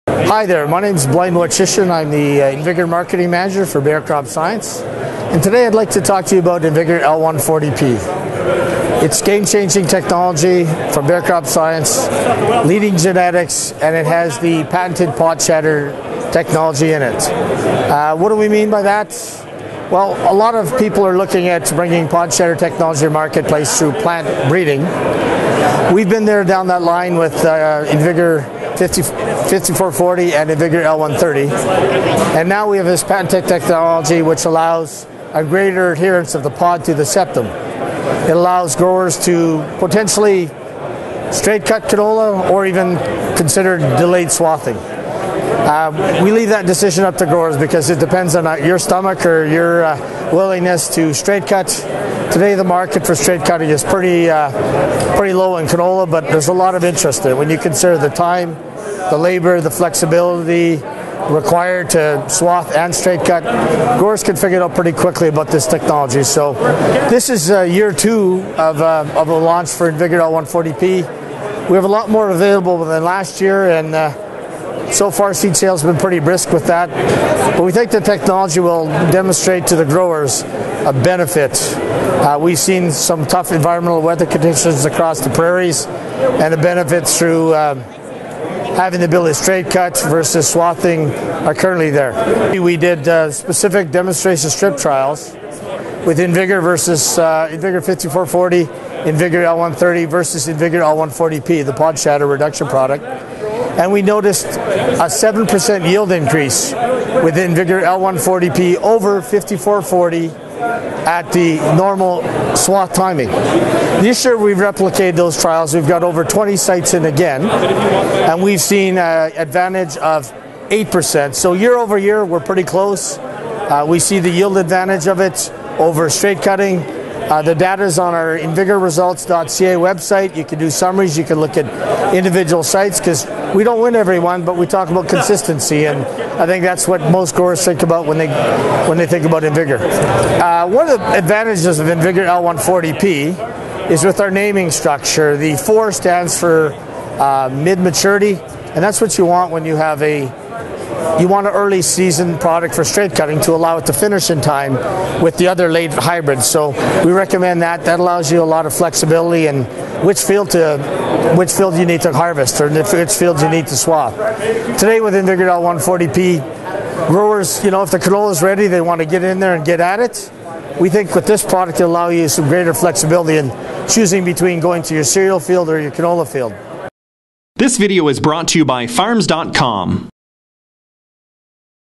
New Bayer CropScience InVigor L140P Pod Shatter Technology
Bayer Crop Science InVigor L140P With New Pod Shatter technology reduces the shattering.   This unique innovation offers excellent canola yield protection and harvest flexibility including the ability to delay swathing or straight cut your canola crop.